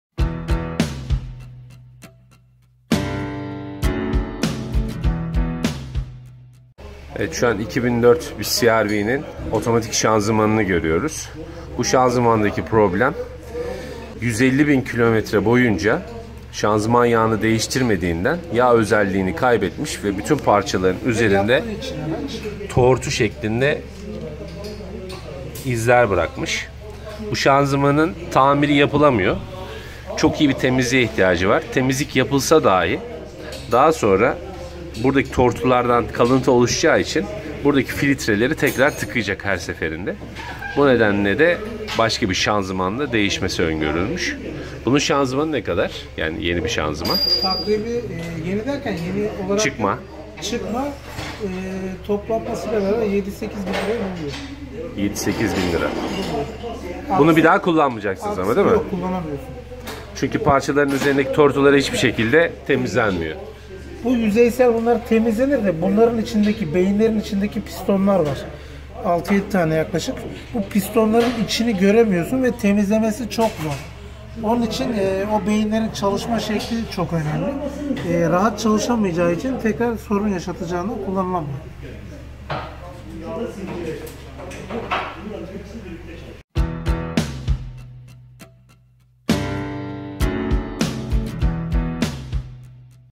HONDA CRV ŞANZIMAN YAĞINI 150.000 KM DEĞİŞMEZSENİZ NE OLUR (crv automatic transmission failure )
CRV ŞANZIMAN ARIZASINI ELE ALDIK.